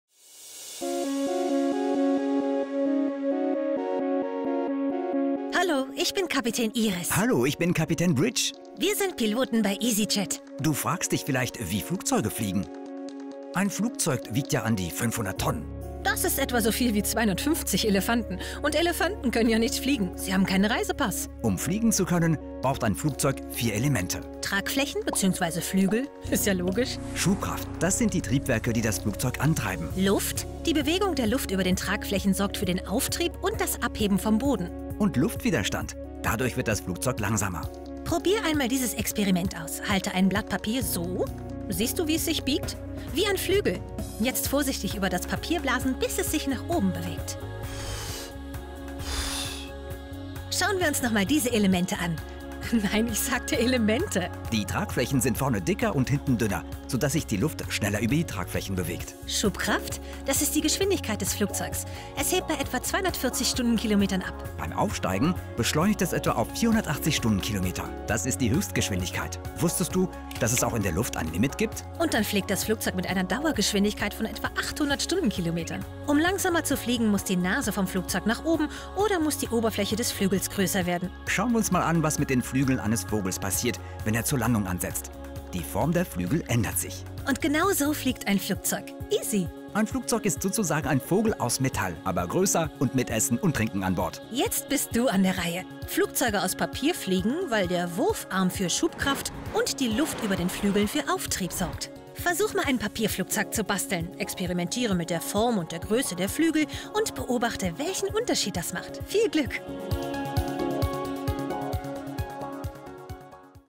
easyJet Junior Flugschule #1 Wie Flugzeuge fliegen
Mit der Junior Flugschule von easyJet wird es dir und deinen Kindern während des Lockdowns garantiert nicht langweilig! ✈️ Unsere Pilotinnen und Piloten erklären kinderleicht wie Flugzeuge fliegen, warum das Wetter wichtig ist und vieles mehr. Schau dir unsere Videos an und werde ein echter Flugexperte.